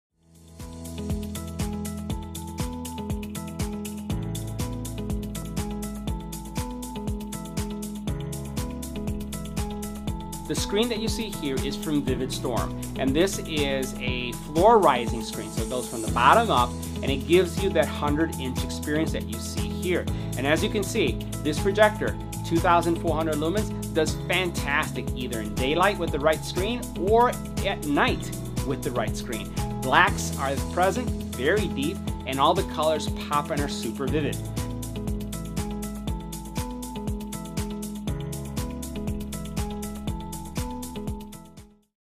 JMGO x Vividstorm ロマン&究極のホームシアター【映画館に行かなくなる】自宅で映画館！
クリスマス割引10％
昼でも楽しめる！魔法のプロジェクタースクリーン（VIVIDSTORM 超短焦点レーザープロジェクター専用 環境光シャットアウトスクリーン）